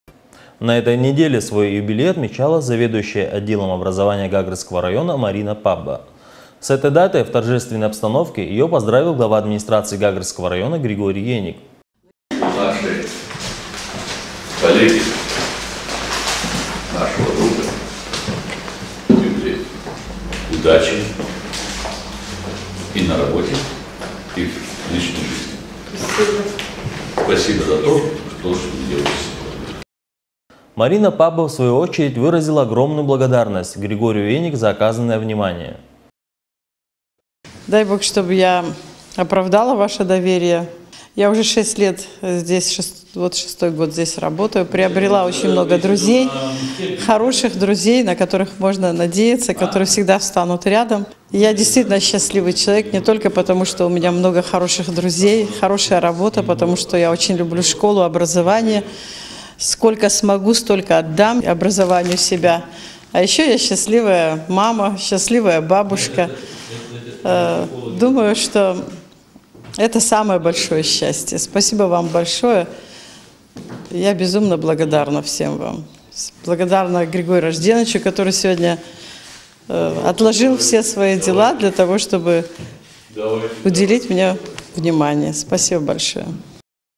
Юбилей Марины Папба
Свой юбилей отмечала заведующая отделом Образования Гагрского района Марина Папба. С этой датой в торжественной обстановке ее поздравил Глава Администрации Гагрского района Григорий Еник.